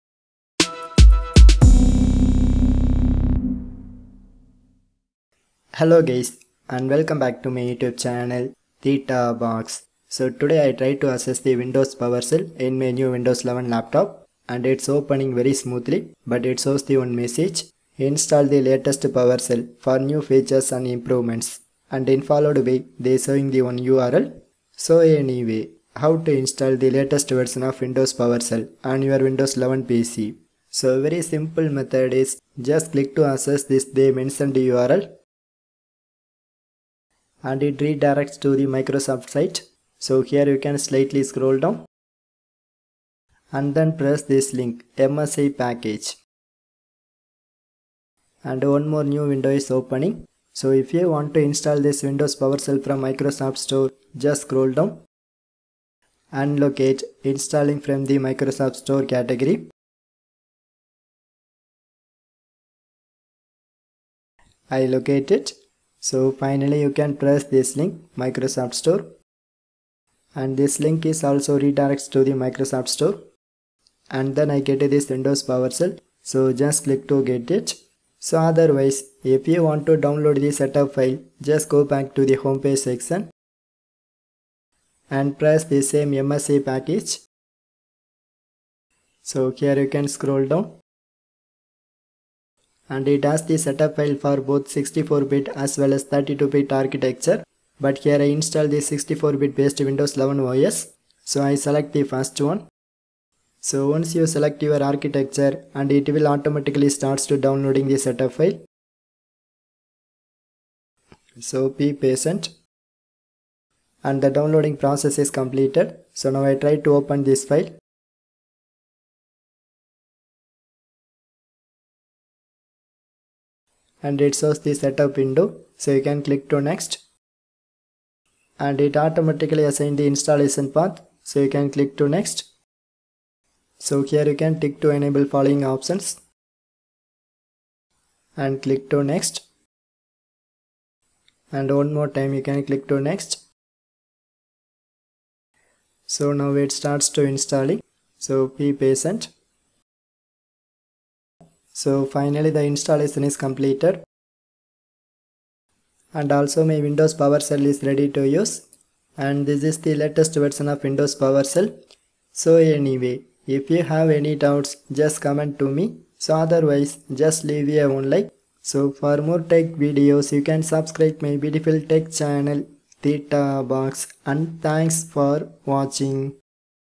How to install latest version of powershell in Windows 11 | Upgrade Powershell to 7.2.0
This tutorial is about how to update windows powershell 7.1 to 7.2.0 in windows 11.

By using this way i update my old windows powershell to 2023 released latest version of 7.2.

There is no need to uninstall that older version of powershell.

This solution also works on windows 7,windows 10,windows server 2012 r2 and windows 2008 r2.

If you use following versions like 4.0 or 5.1,7.1.2,7.1.3,7.1.4 and 7.1.5 may be not working.

How to install latest powershell in windows 11 :

1.One of my subscriber ask how do i upgrade my powershell 5.1 to 7.2.0?.

2.This is very simple,just click to access they mentioned url.

3.In home page section you can press "MSI package".

4.If you want to install this windows powershell from microsoft store,

5.Just locate "Installing from the Microsoft Store" section.

6.Here you can press that "Microsoft Store" link and then your store is opening.

7.Finally you can click to "Get" option and it starts to downloading.

8.At same time if you want to download this setup file,just goto "MSI package".

9.Here it shows the setup file for both 64 bit as well as 32 bit.

10.So you can choose your windows architecture x86 or x64.

11.Once the downloading is completed,just click to install it.